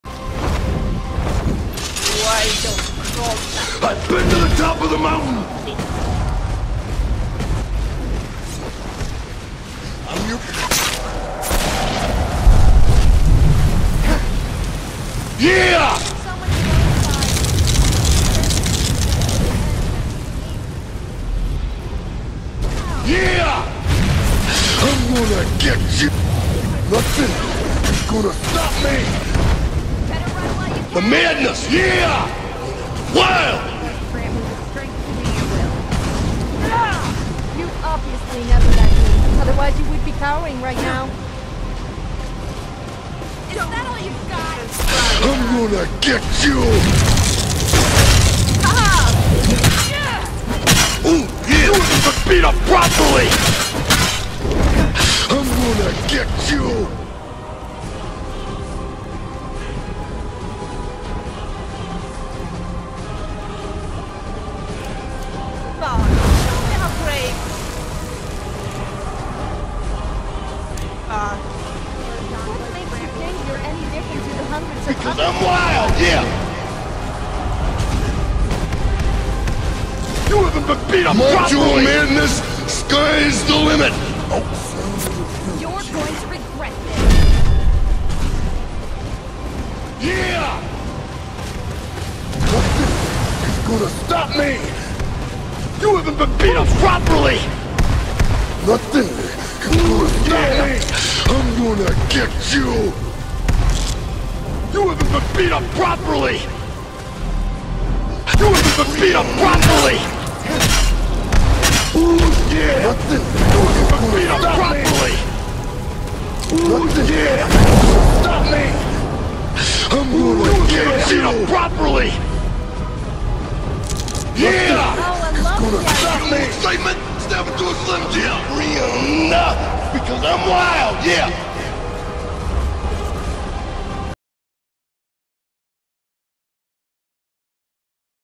SNAP INTO A SLIM JIM!
Messing around with the Randy Savage dragon mod a friend told me about.